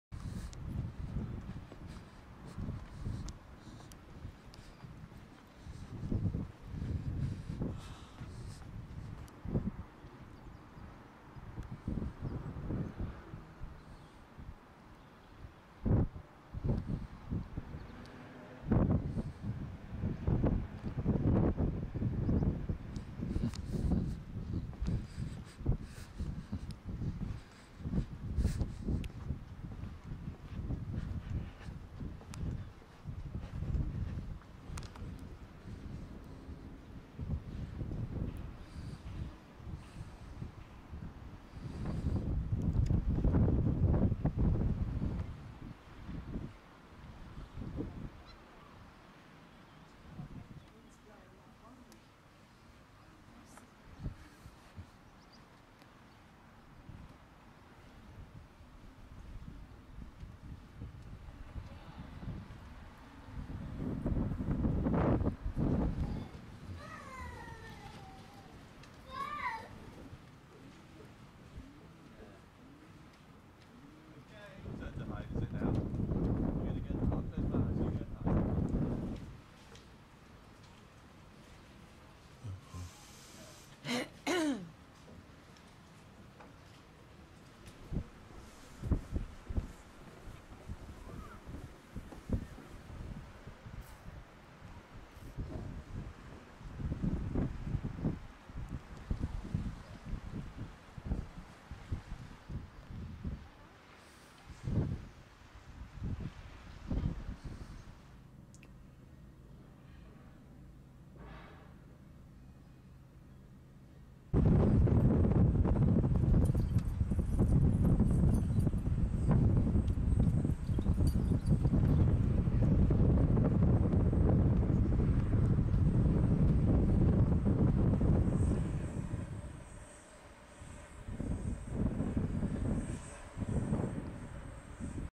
Outings from Pontins, Sand Bay - Clevedon Pier, Spring 2019
It's just half an hour from Western Super Mare, which makes Clevedon Pier an easy trip with your own car. The town of Clevedon is quiet and clean, and the Pier itself is unusual in design, and has an interesting history.
It's just £3.00 for adults and £2.50 for pensioners in 2019.